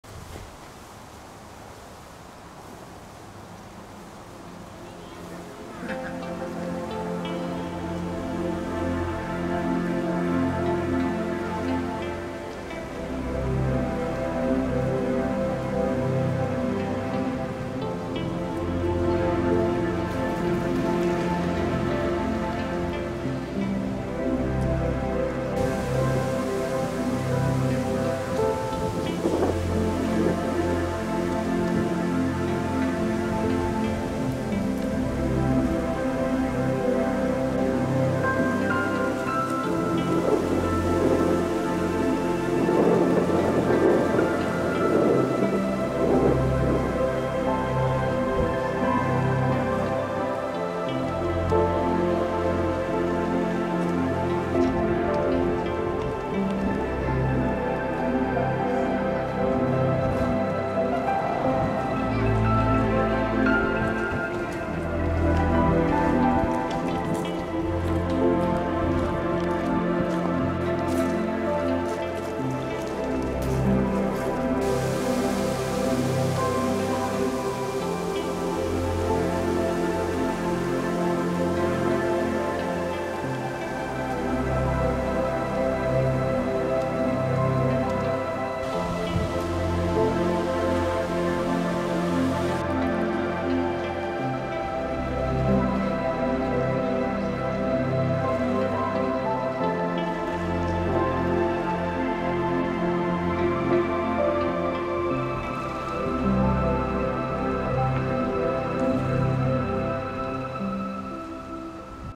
20201128 群馬県紅葉情報 高崎 少林山達磨寺
いよいよ、紅葉シーズンの最終章となりました。高崎達磨寺をUPします。あと、数日は楽しめそうです。参考になれば幸いです。